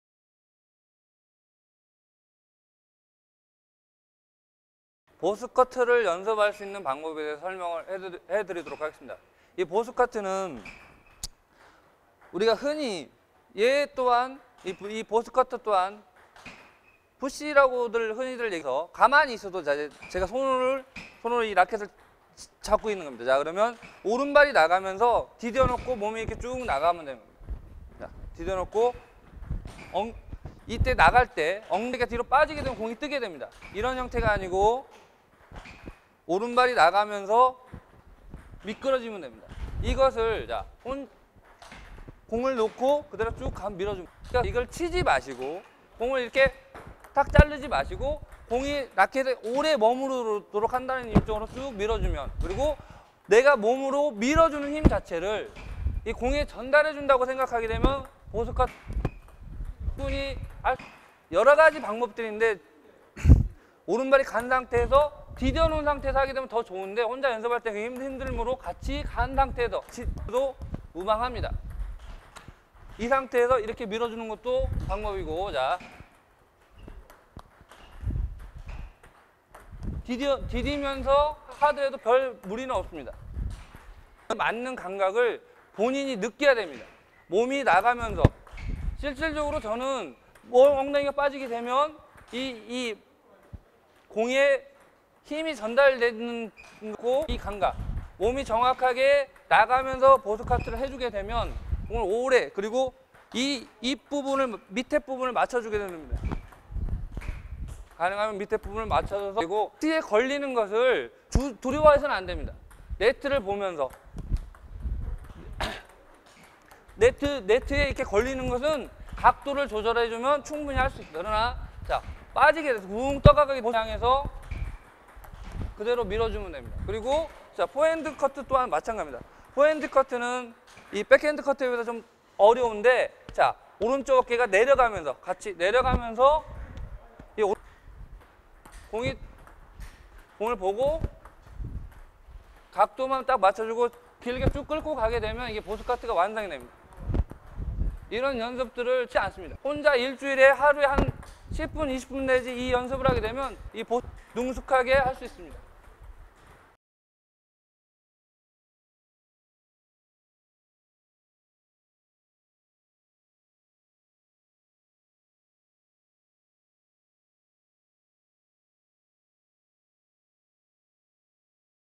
탁구누리 레슨 백핸드 보스커트 및 포핸드 보스커트 혼자하는 연습방법
탁구누리 백핸드 보스커트 및 포핸드 보스커트 혼자하는 연습방법